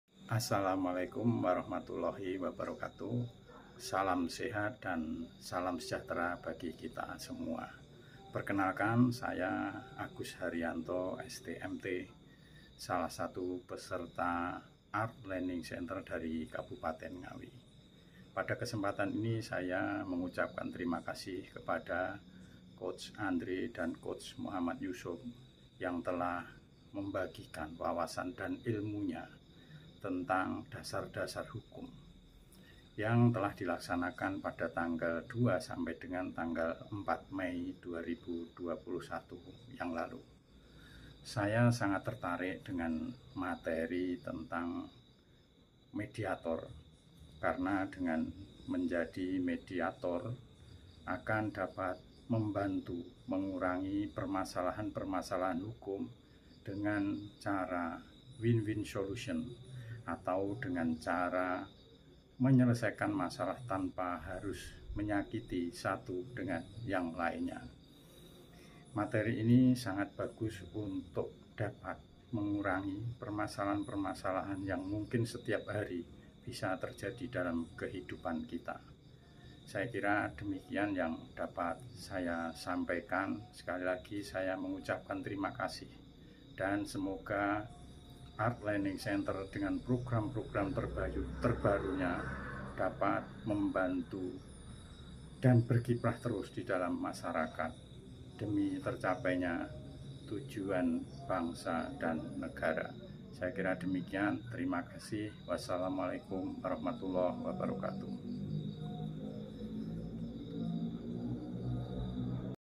Video Review Kelas Sertifikasi ALC Fundamental Law Science Batch 1 Oleh Agus Hariyanto || Ngawi
Tugas 2 Video Review Kelas ALC Certified Fundamental Law Science (C.LSc) Batch 2

Oleh Agus Hariyanto

Asal Ngawi